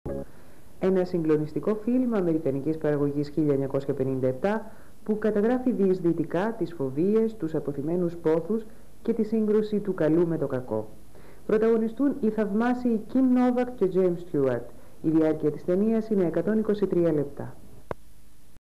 ΔΑΦΝΗ ΜΠΟΚΟΤΑ ΕΤ 1
Αναγγελία προγράμματος από την τηλεπαρουσιάστρια της ΕΤ-1 Δάφνη Μπόκοτα.